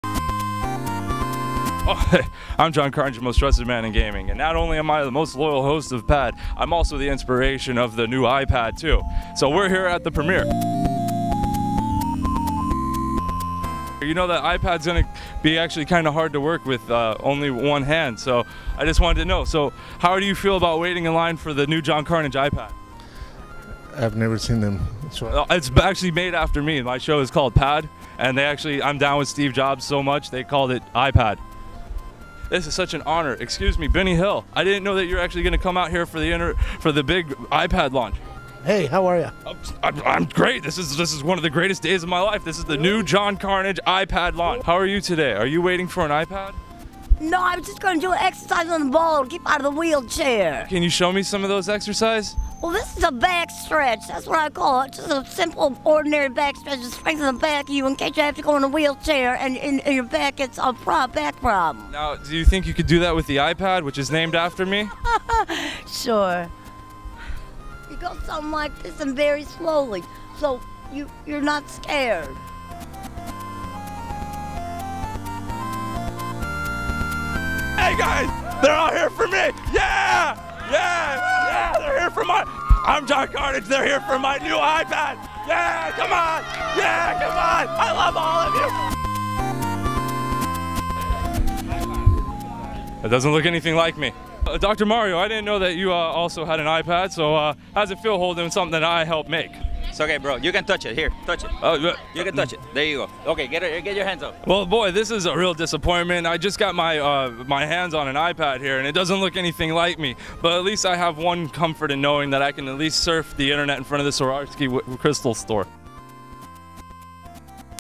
P.A.D.-IPAD
JonCarnage goes to pick up his new I P.A.D. named after his show.
I like so many others was superly-duperly excited for the new iPad launch! Not just because its got really neat features like soaking up 12x's its weight in liquid.  But it's also named after my show P.A.D.! You know, me and that Paul Jobs are really good mates. After hearing the big news that my show was now a super high tech piece of equipment.I decided to headed over to South Beach and talk amongst the thousands of people waiting in line to get their hands and multiple fingers on it.